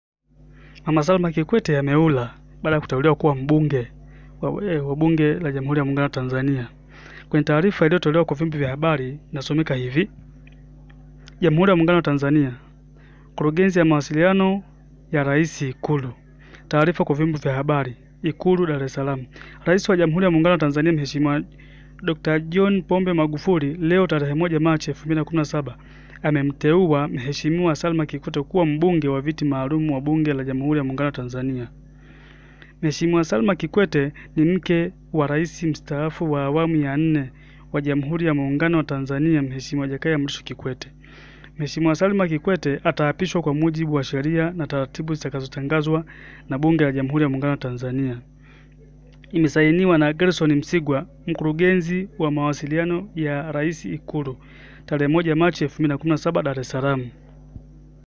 Mama Salma Kikwete ateuliwa kua Mbunge
Aliyekua first lady wa serikali ya awamu ya nne mama Salma Kikwete ameteuliwa kua mbunge wa bunge la jamhuri ya muungano wa Tanzania